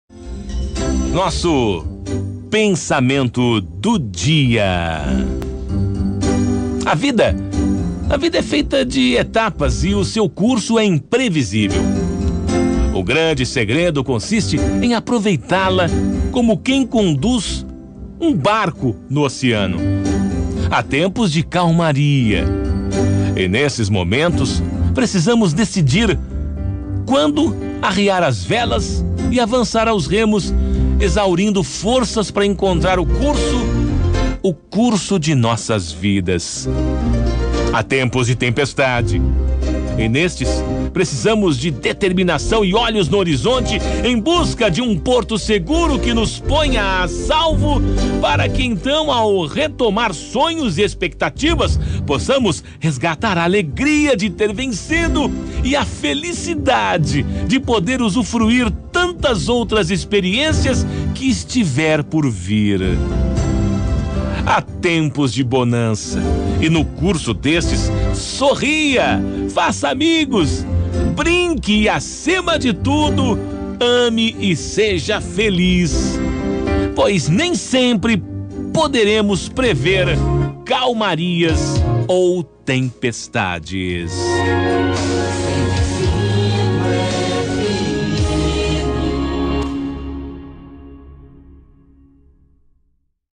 A VIDA É FEITA DE ETAPAS - Pensamento do dia
Pensamento do dia com Paulinho Maurício - 31/03/21

A VIDA É FEITA DE ETAPAS E O SEU CURSO É IMPREVISÍVEL. O GRANDE SEGREDO CONSISTE EM APROVEITÁ-LA COMO QUEM CONDUZ UM BARCO NO OCEANO.

HÁ TEMPOS DE CALMARIA, E NESTES MOMENTOS PRECISAMOS DECIDIR QUANDO ARRIAR AS VELAS E AVANÇAR AOS REMOS EXAURINDO FORÇAS PARA ENCONTRAR O CURSO DE NOSSAS VIDAS.

HÁ TEMPOS DE TEMPESTADE, E NESTES PRECISAMOS DE DETERMINAÇÃO E OLHOS NO HORIZONTE EM BUSCA DE UM PORTO SEGURO QUE NOS PONHA A SALVO, PARA QUE ENTÃO, AO RETOMAR SONHOS E EXPECTATIVAS, POSSAMOS RESGATAR A ALEGRIA DE TER VENCIDO, E A FELICIDADE DE PODER USUFRUIR TANTAS OUTRAS EXPERIÊNCIAS QUE ESTIVER POR VIR.

HÁ TEMPOS DE BONANÇA, E NO CURSO DESTES, SORRIA, FAÇA AMIGOS, BRINQUE E ACIMA DE TUDO, AME E SEJA FELIZ! POIS NEM SEMPRE PODEREMOS PREVER CALMARIAS OU TEMPESTADES.